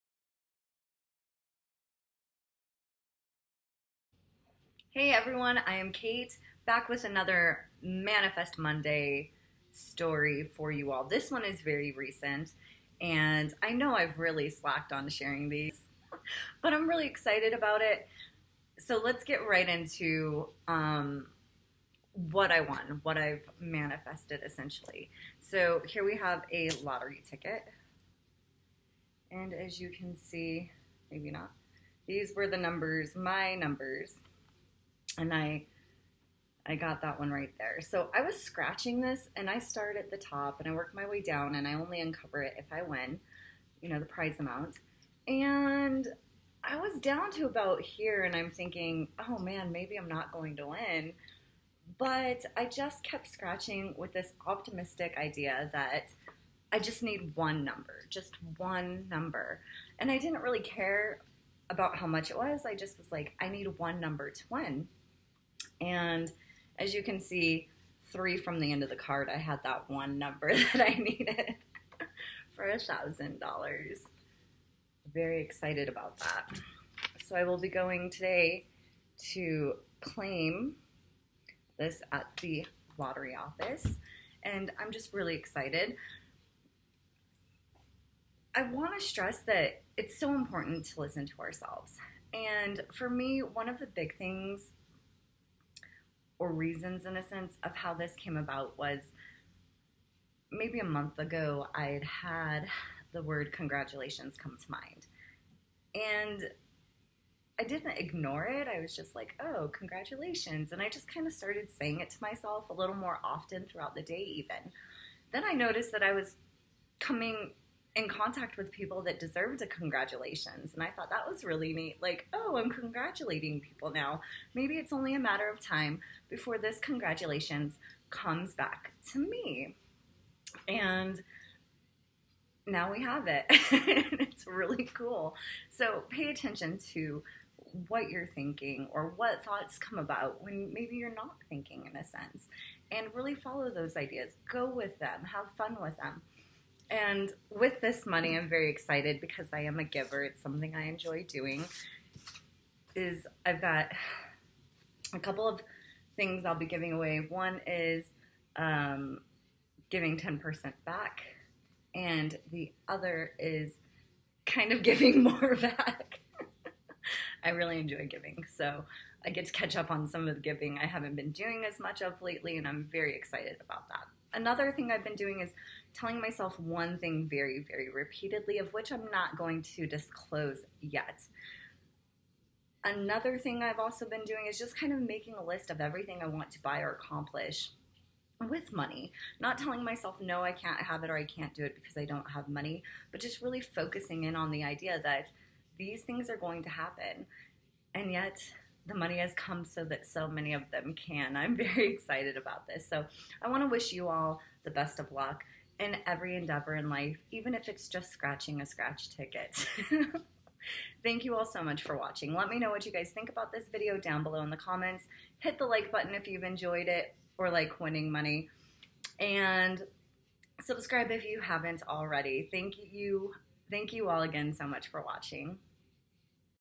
Manifest Monday $1000 WINNER!!!
A little info on my most current winnings of  $1000.00.

Enjoy, and BEST of LUCK/BLESSINGS to you all.